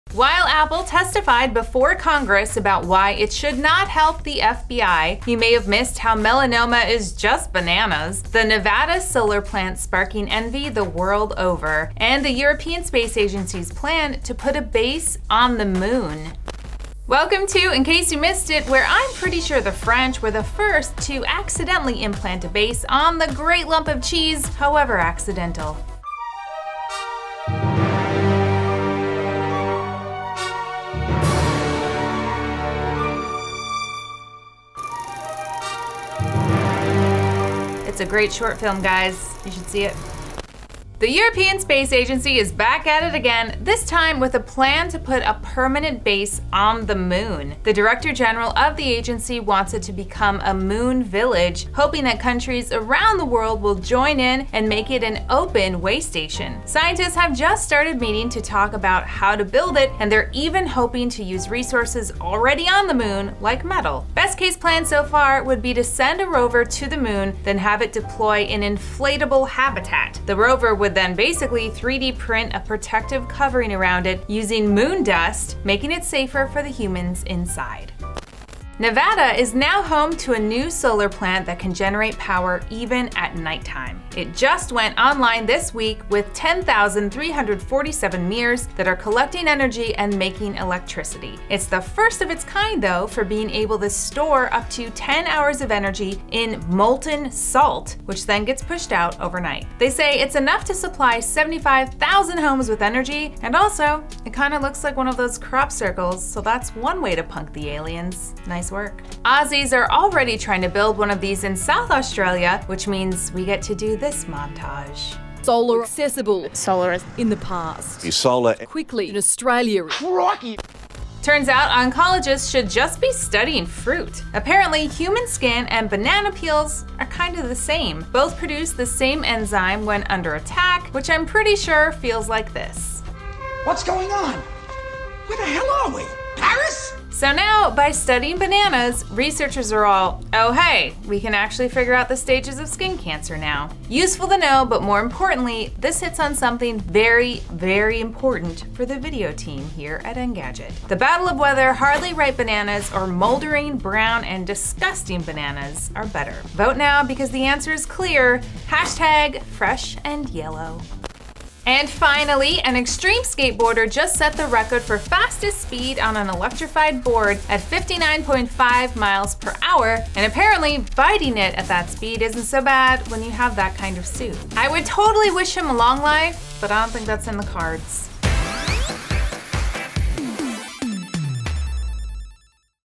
ICYMI: Moon waystation, human skin like bananas, and more
Today on In Case You Missed It: The European Space Agency wants to put a permanent base on the moon, for all agencies to share. A new solar plant in Nevada is the first to store 10 hours of energy in salt, continuing to produce electricity overnight. And researchers are studying bananas because an enzyme in their skin is just like that of human skin. Maybe that’s why they taste so good? JK, that was just for Hannibal Lecter.

We also wanted to bring you this latest Guinness World Record, of an electrified skateboard traveling at 59.5 miles per hour. As always, please share any interesting science or tech videos, anytime!